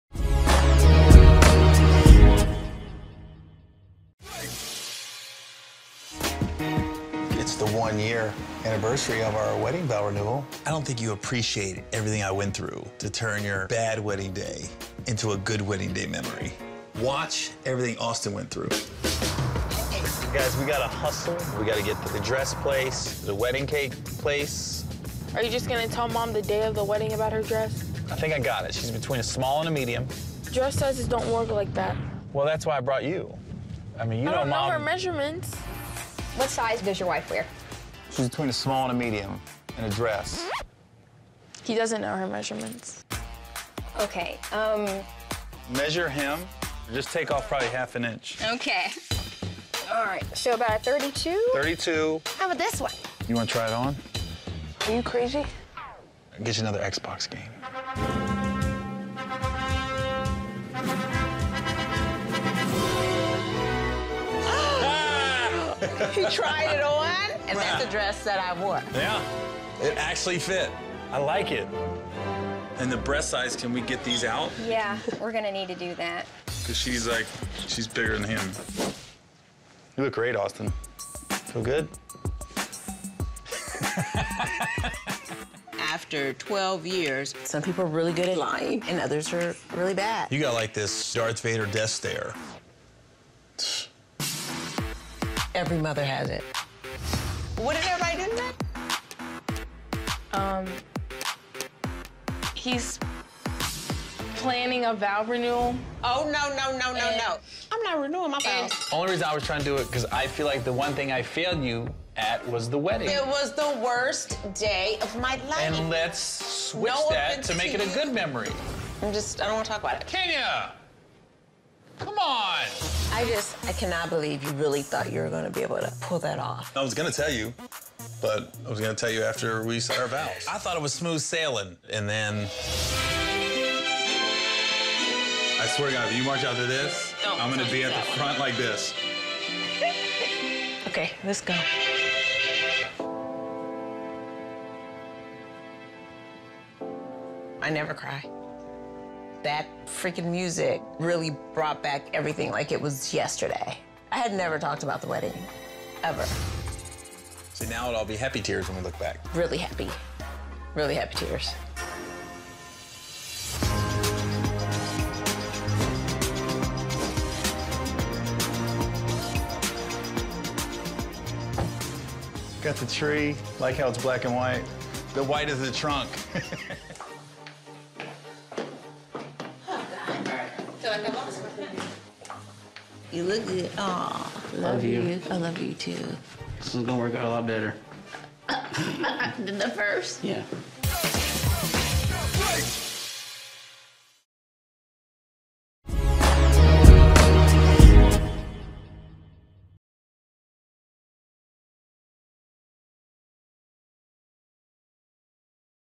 See Why Gary Made His Son Put on a Dress
Gary went to great lengths to correct his biggest regret in life.